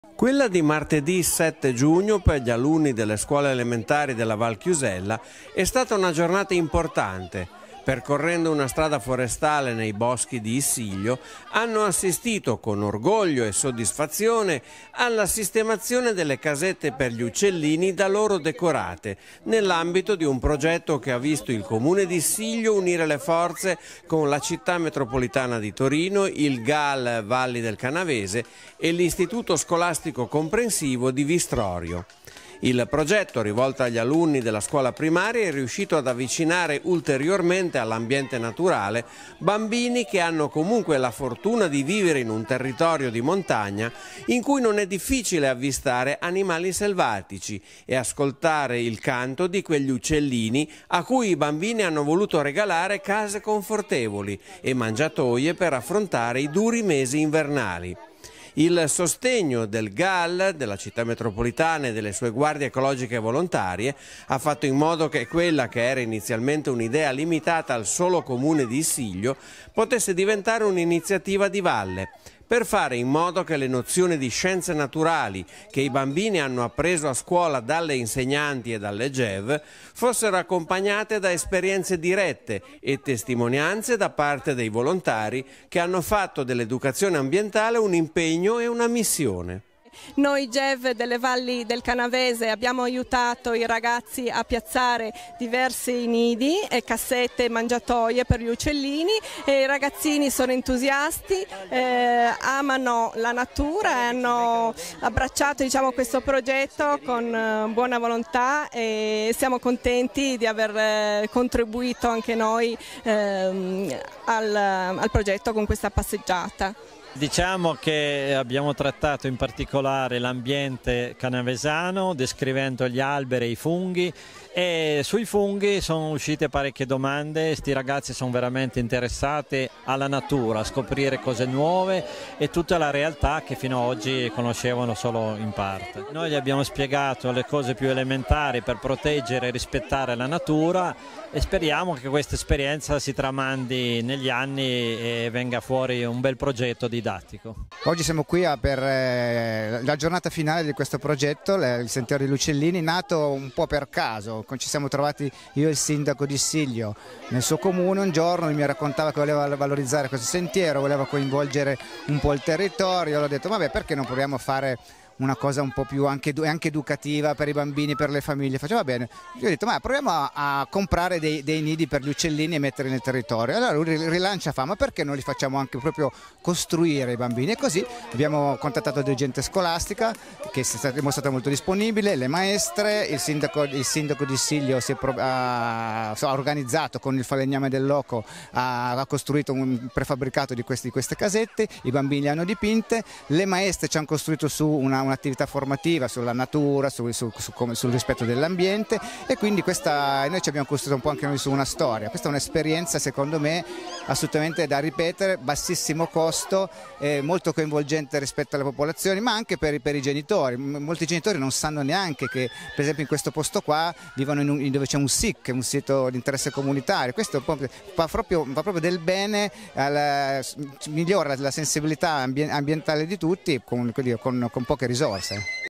Le casette degli uccellini, sugli alberi della Val Chiusella
Martedì 7 giugno, si è concluso il progetto di sensibilizzazione ambientale che ha coinvolto i bambini delle scuole della Val Chiusella, con la sistemazione sugli alberi delle casette per gli uccelli costruite dai bambini.
Il progetto ha coinvolto  il Comune di Issiglio, la Città metropolitana di Torino, il GAL Valli di Canavese e l'Istituto scolastico comprensivo di Vistrorio.